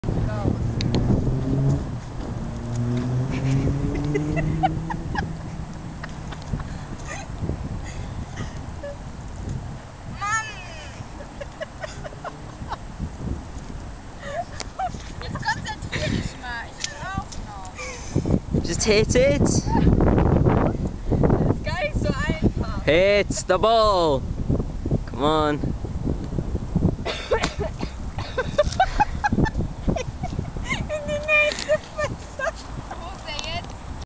zitat: "hit the ball" xD xD xD
kathas golfball ist in die pfütze geflogen und jetzt muss man den auch wieder rausbekommen..XD das video ist nach 5 minuten aufgenommen worden..xD